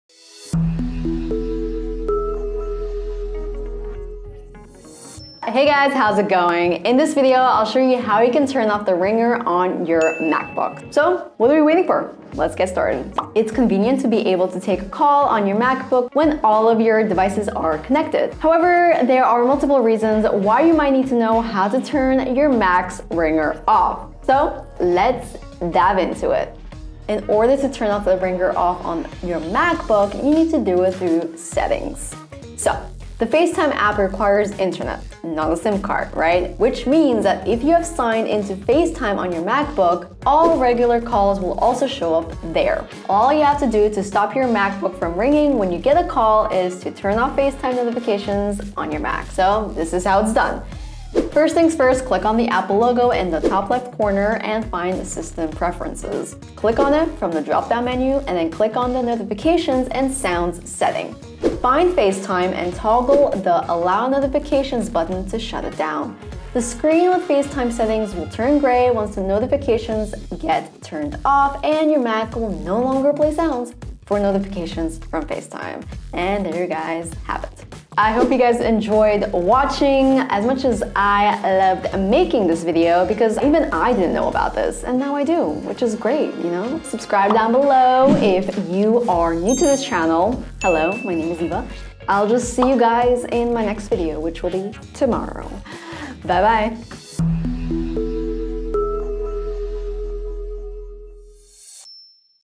How to Turn Ringer Off on MacBook
It's convenient to be able to take a call on your MacBook when all of your devices are connected. However, there are a variety of reasons why you might need to know how to turn your Mac's ringer off. In this video, we'll show you how to turn off call alerts on your Mac.